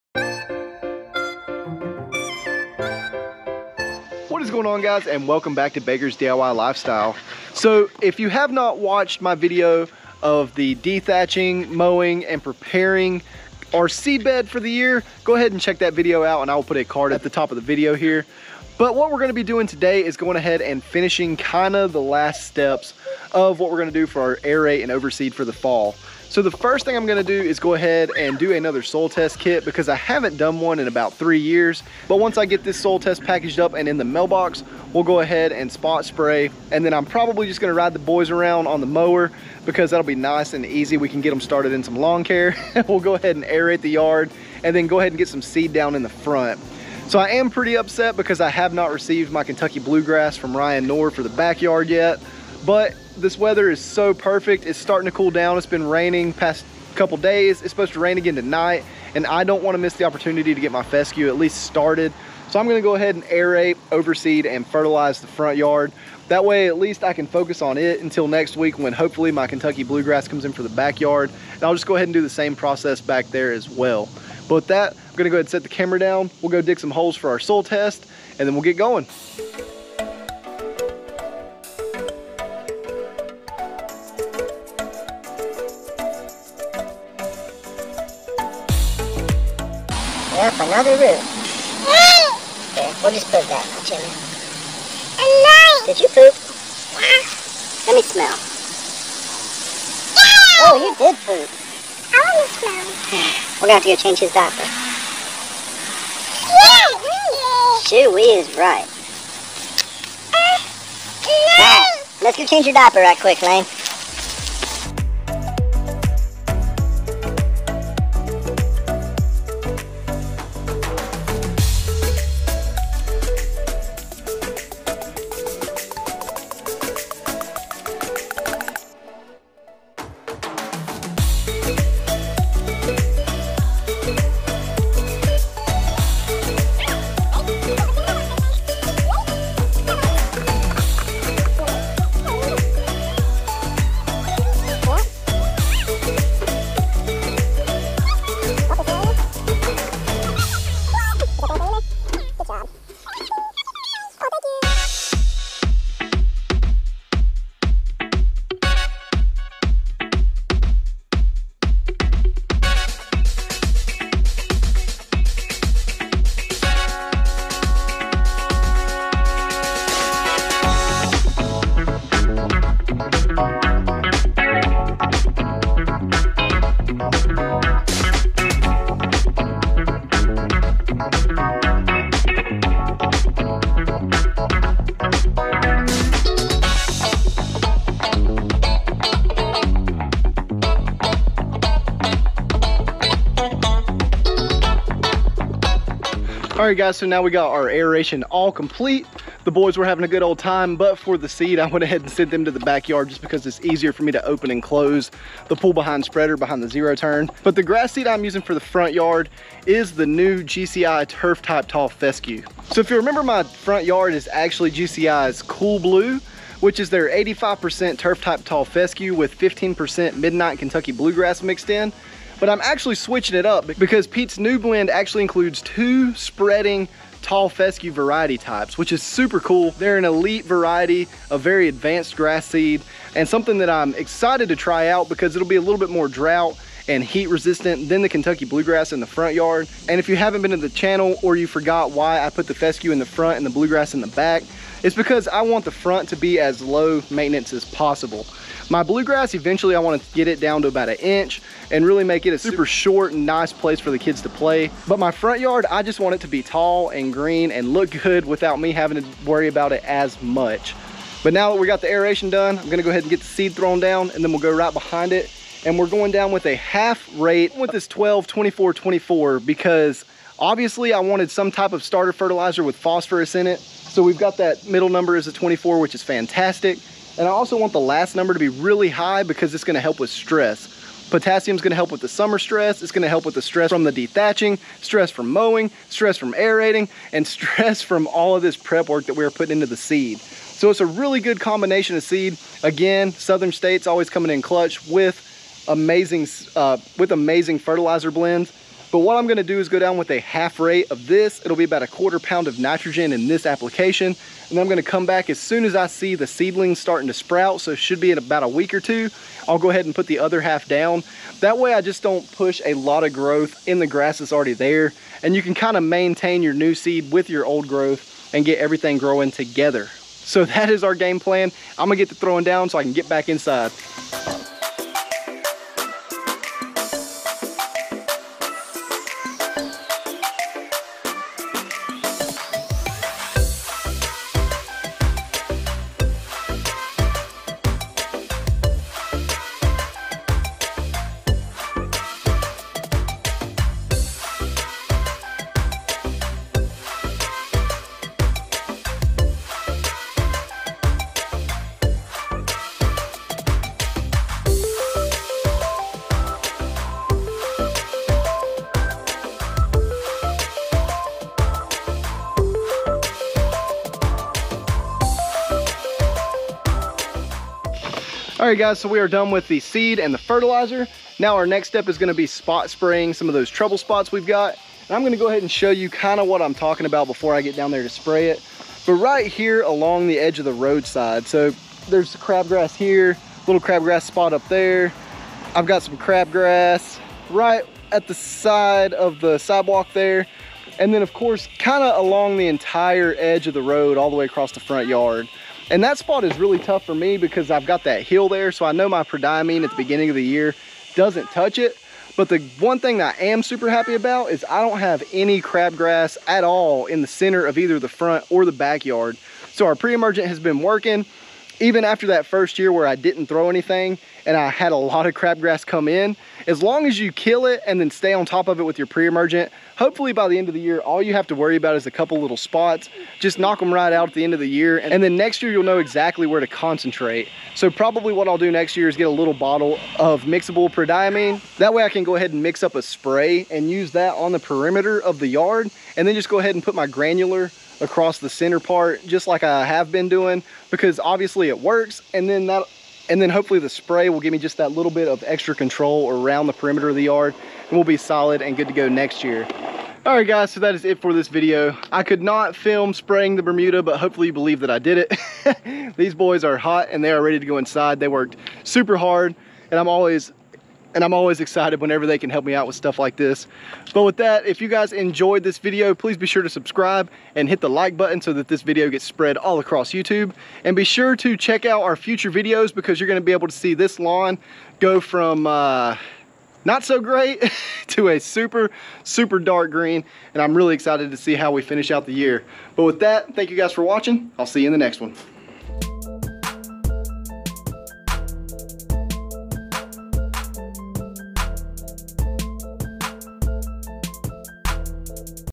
GCI TURF overseed | AERATE and OVERSEED | SOIL TEST | Fall LAWN CARE
Today we are going to be repairing our damaged cool season lawn and bringing it back to glory before the end of the growing season. We will be throwing our GCI turf type tall fescue with a dose of starter fertilizer to get the lawn rejuvenated and ready to fill in.